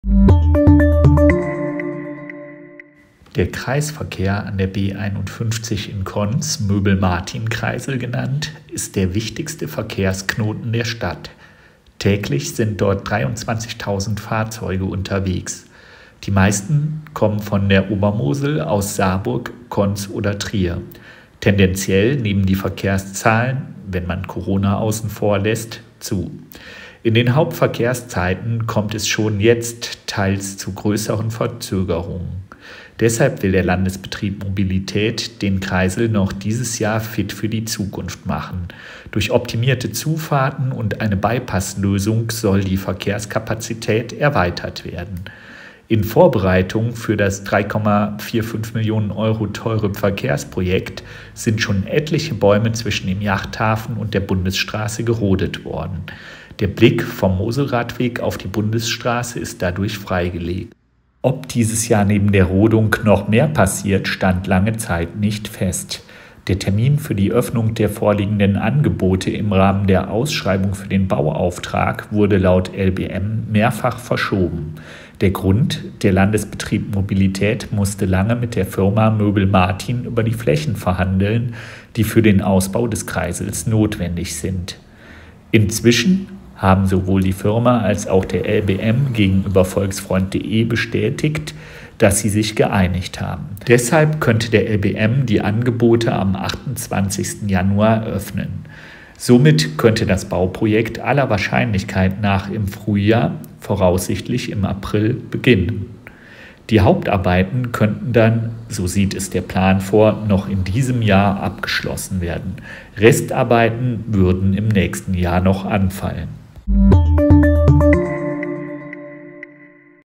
Konz bekommt einen Turbokreisel an der B 51
Arbeiten am wichtigsten Verkehrsknotenpunkt in Konz. Nach langwierigen Verhandlungen sieht es so aus, als könnte der Bau des Turbokreisels an der B 51 in Konz wie geplant in diesem Jahr beginnen.